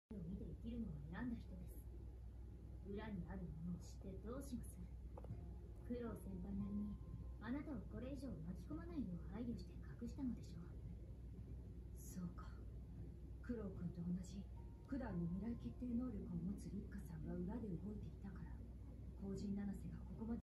子犬が寝るところ🐶ラブラドゥードゥル
ククルが来たばかりの頃なので
4ヶ月くらいの時です😊

子犬って直前まで元気に遊んでいても
すぐに電池が切れてしまうところが
すごく可愛い！

嬉しいです！！🐶💛